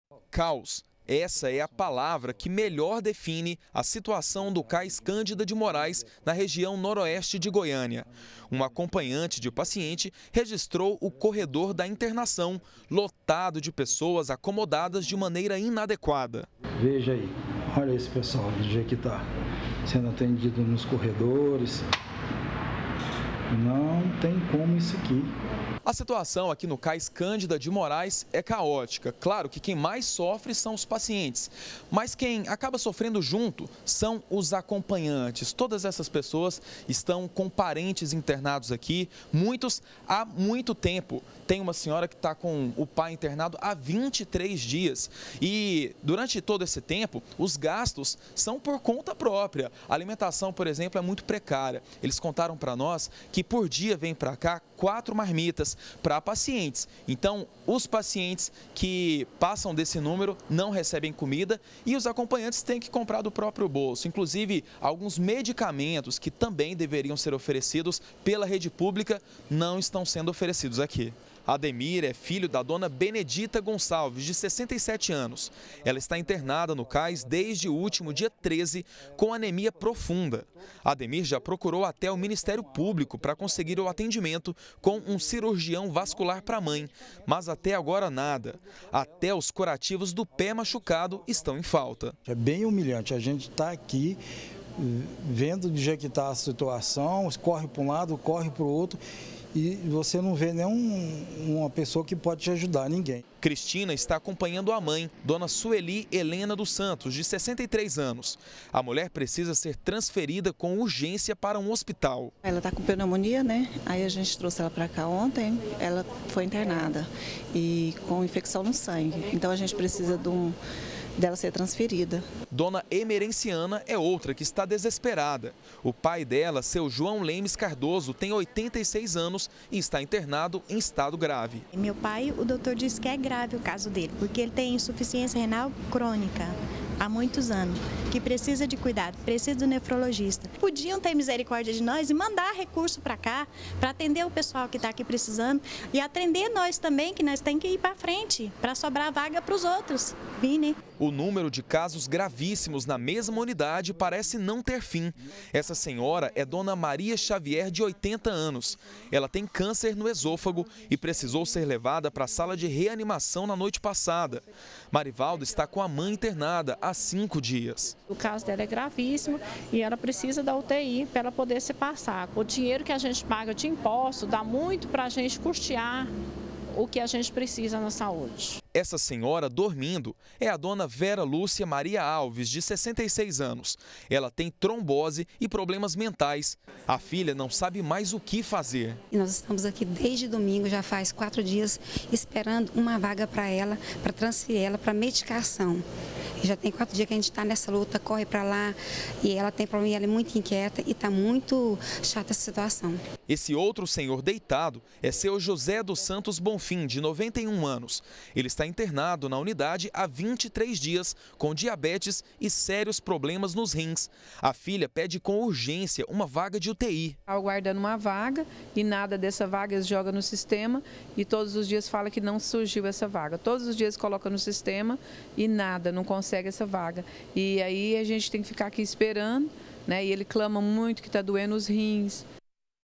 GA - Superlotação no CAIS Cândida de Moraes - 27-09-2017
Superlotação no CAIS Cândida de Moraes. Muitos pacientes dormem nos corredores e os parentes tem que levar comida e remédios, enquanto muitos esperaram por uma vaga de UTI.